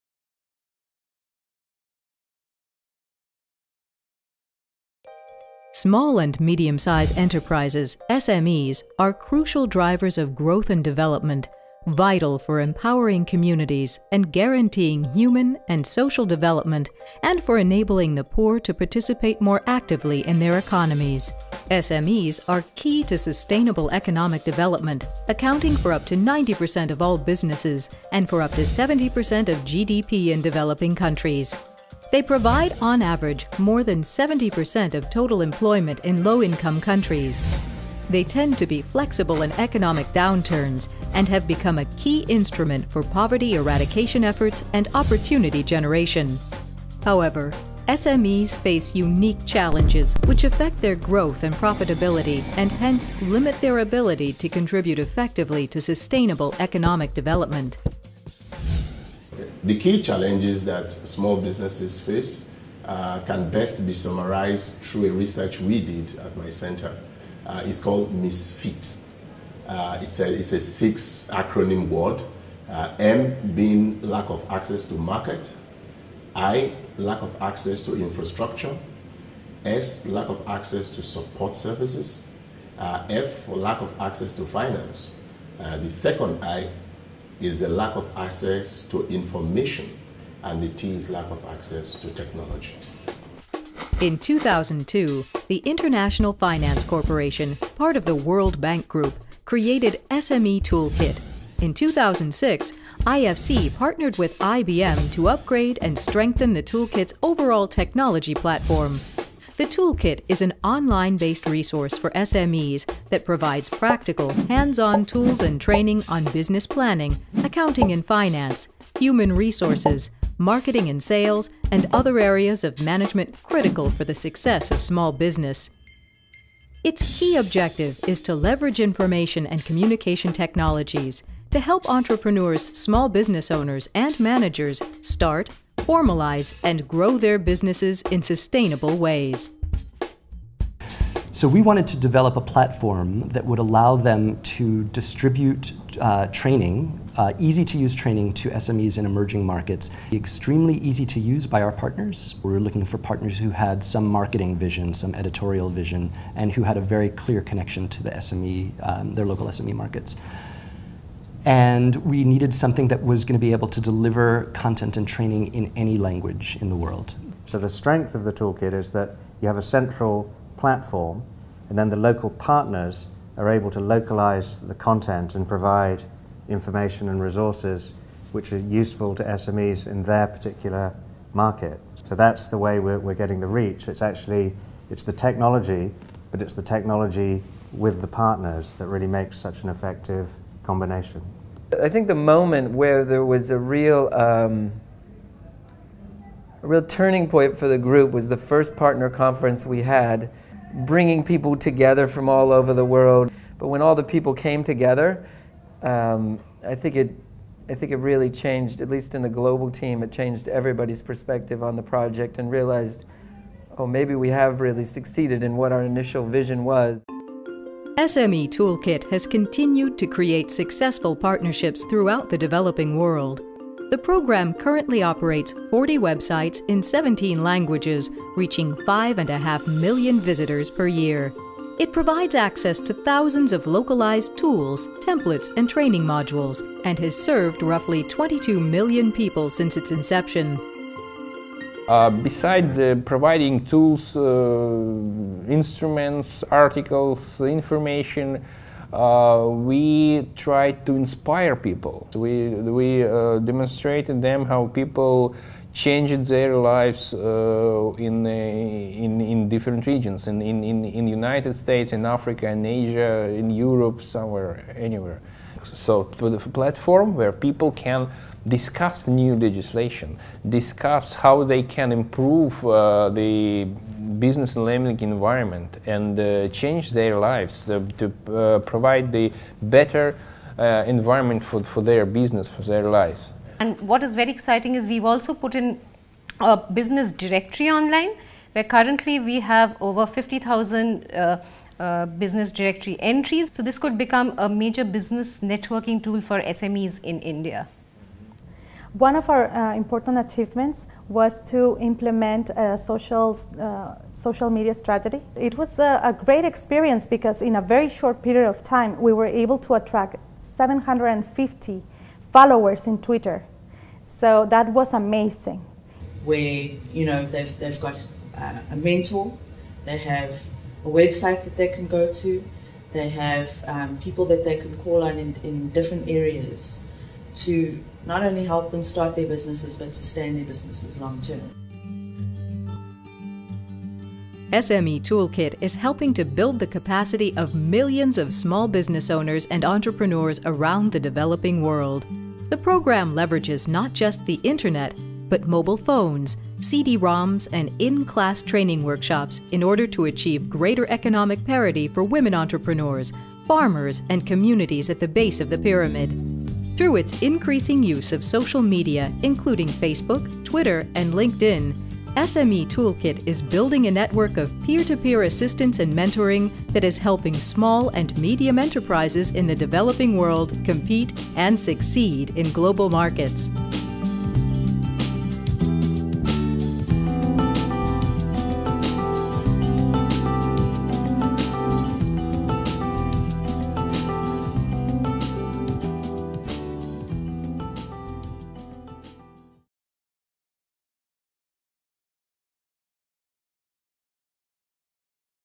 Strengthening small businesses in emerging countries - Stories of SME Toolkit
The SME Toolkit is a tool designed by the International Finance Corporation (IFC), a member of the World Bank Group, with support of IBM, with the aim of using the latest information and communication technologies to help small and medium enterprises in emerging markets learn sustainable business management practices.
Small businesses are the growth engines of the world's economies; yet their success rate is not as good as it could be simply because of a lack of access to good business management practices.

The SME Toolkit is providing small businesses the information and new collaborative technologies they need, in order to help them grow and prosper.

Produced by: IFC SME Toolkit and IBM
A Video by CAD Productions

Director: Fernando Casado Cañeque
Photography and Edition Direction: Antonio Rodrigo Paulete
Editing support: Vision Films: Ramon Vila and Oscar Mibelli
Content Advisor: Johanna Klein
Graphic Design by Iosphera:
Vanessa Knorst
Elzine Aristide
Alix de Roten